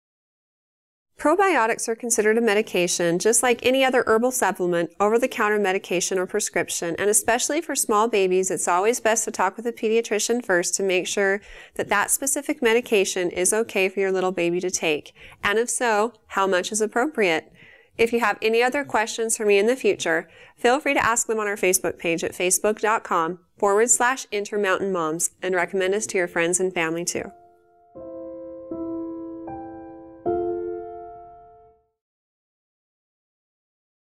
Can I start my 6 week old on probiotics?
Probiotics are considered a medication just like any other herbal supplement, over-the-counter medication, or prescription. And especially for small babies, it's always best to talk with the pediatrician first to make sure that that specific medication is okay for your baby to take, and if so, how much is appropriate. If you have any other questions for me in the future, feel free to ask them on our Facebook page at Facebook.com/IntermountainMoms, and recommend us to your friends and family too.